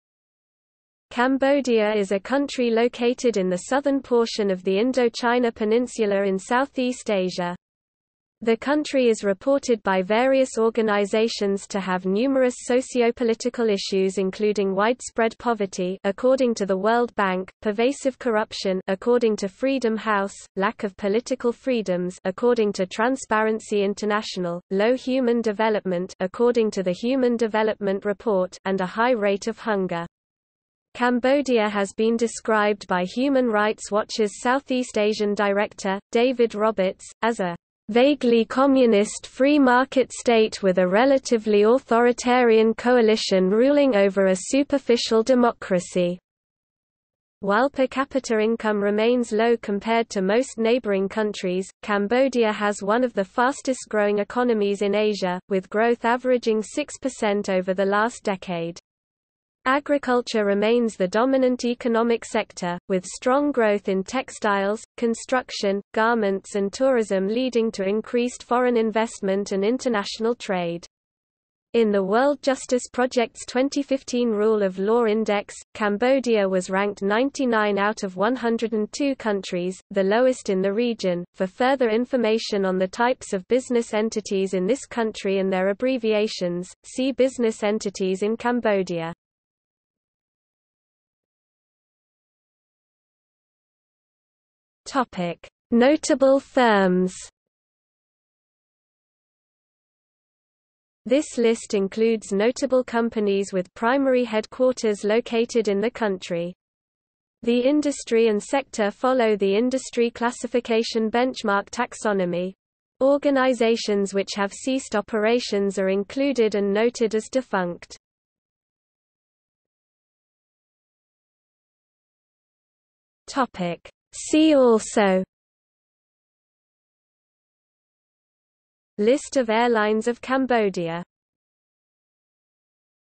List of companies of Cambodia | Wikipedia audio article
00:01:31 Notable firms
00:01:59 See also

- Socrates

SUMMARY
Cambodia is a country located in the southern portion of the Indochina Peninsula in Southeast Asia. The country is reported by various organisations to have numerous sociopolitical issues including widespread poverty (according to the World Bank), pervasive corruption (according to Freedom House), lack of political freedoms (according to Transparency International), low human development (according to the Human Development Report) and a high rate of hunger. Cambodia has been described by Human Rights Watch's Southeast Asian Director, David Roberts, as a "vaguely communist free-market state with a relatively authoritarian coalition ruling over a superficial democracy." While per capita income remains low compared to most neighbouring countries, Cambodia has one of the fastest growing economies in Asia, with growth averaging 6 percent over the last decade. Agriculture remains the dominant economic sector, with strong growth in textiles, construction, garments and tourism leading to increased foreign investment and international trade. In the World Justice Project's 2015 Rule of Law Index, Cambodia was ranked 99 out of 102 countries, the lowest in the region.For further information on the types of business entities in this country and their abbreviations, see Business entities in Cambodia.